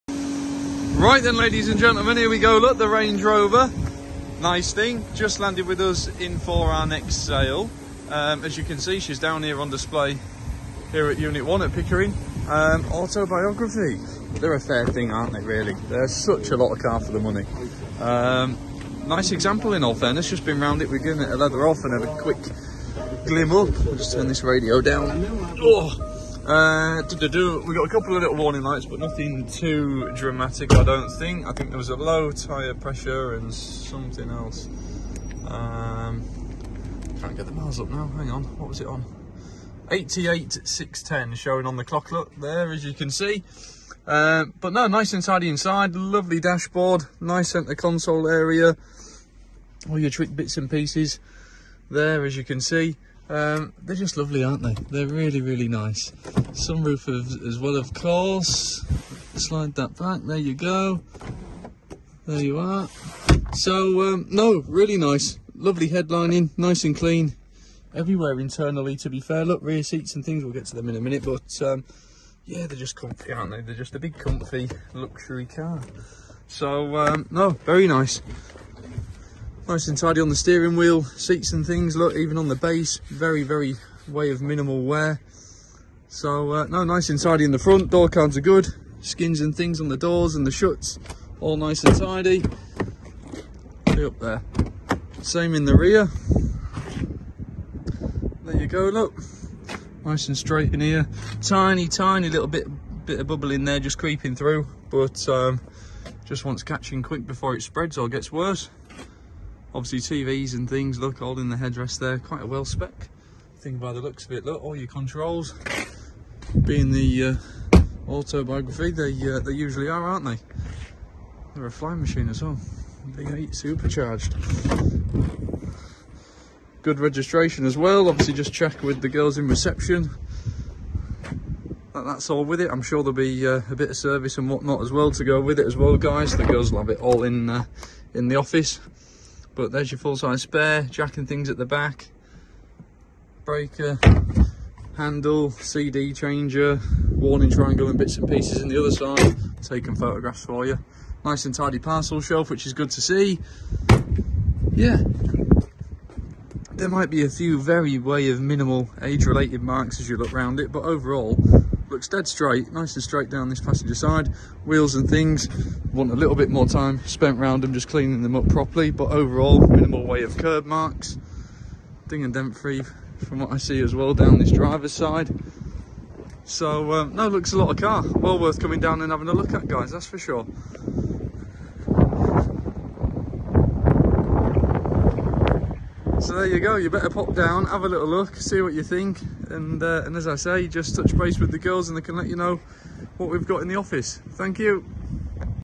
2009 LAND ROVER RANGE ROVER AUTOB S/CV8 A | MATHEWSONS CLASSIC CARS | AUCTION: 1, 2 & 3 MAY 2024
2009 LAND ROVER RANGE ROVER AUTOB S/CV8 A | MATHEWSONS CLASSIC CARS | AUCTION: 1, 2 & 3 MAY 2024

Join Derek, Dave, Paul and everyone at Mathewsons Classic Cars for our live online classic car, motorcycle and memorabilia auction on May 1, 2 & 3 2024.